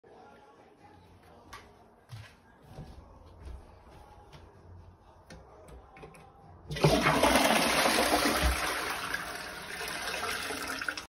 How to flush a toilet bathroom
hello boys and girls today I'm back with another banger of a video I'm showing a video on how you flush a toilet I hope you find this video self-explanatory and I hope you like the video and subscribe and leave a comment if it worked anyways I will be seeing you tomorrow bye

My Snapchat username

trashgangtrix
(cringy name :p)

sasatro92

Thats it 😎